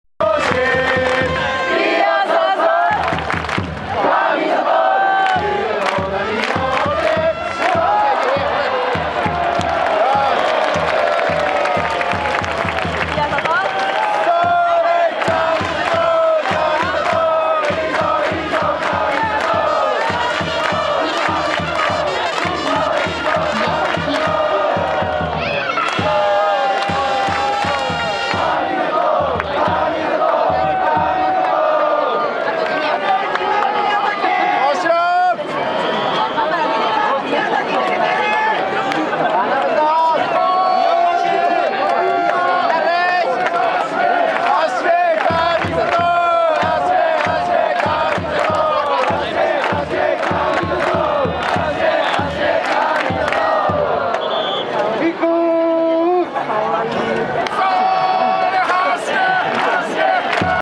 DeNA国吉と砂田のブルペンピッチング映像
横浜DeNAベイスターズ国吉投手と砂田投手のブルペンでのピッチング映像。神宮球場にて撮影。2019年5月19日　対スワローズ戦。